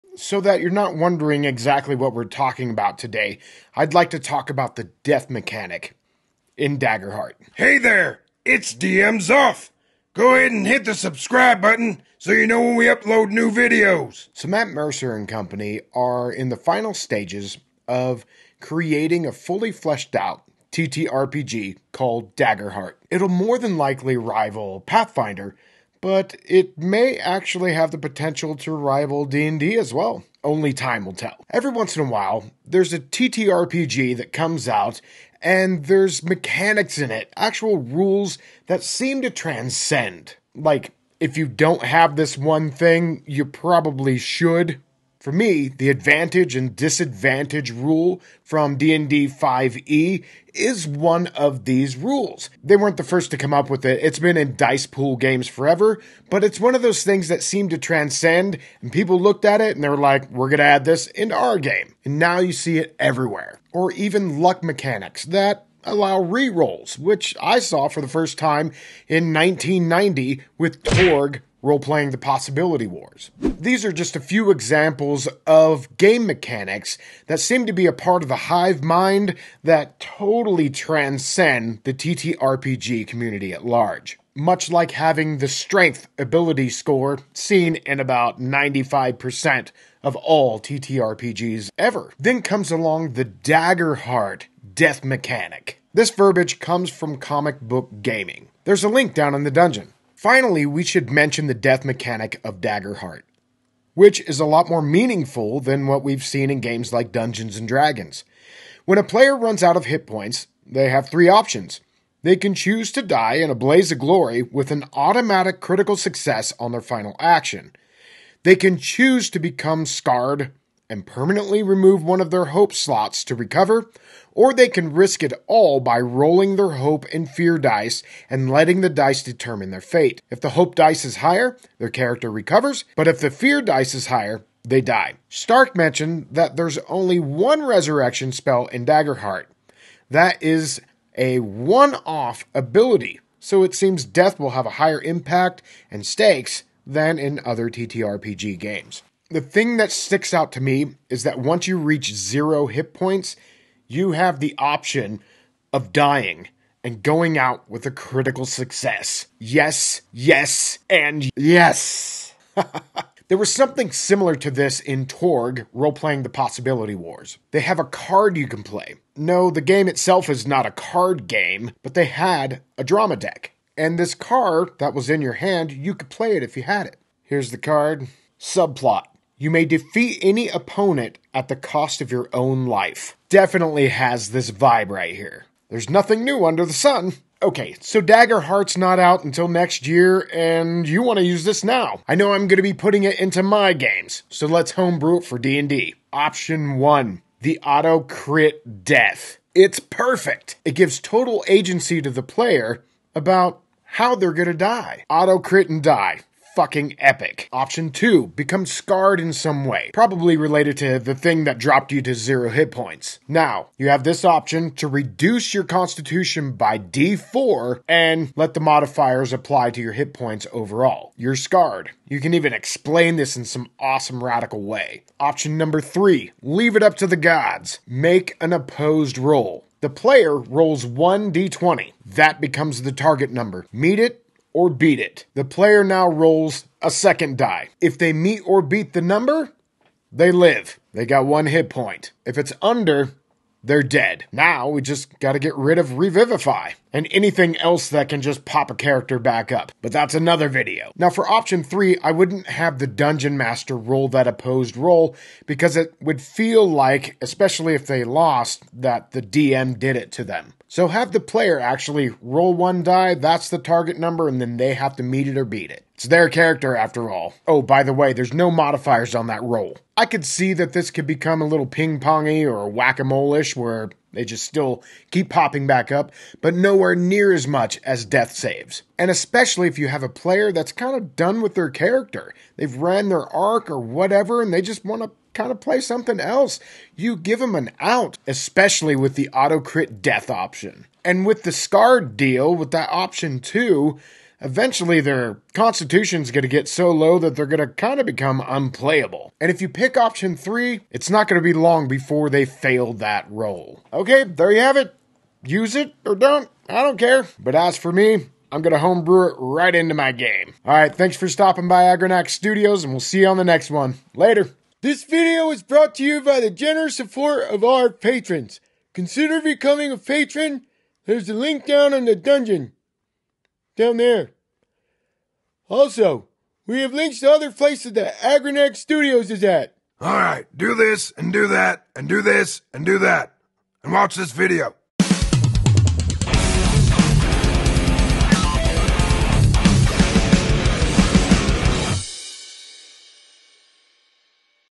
Daggerheart RPG Death Mechanics | Critical Role
DM Wisdom talks about taking the Daggerheart Death Mechanic and putting it into your Dungeons & Dragons game.

=== Where Agranak Studios Is At ===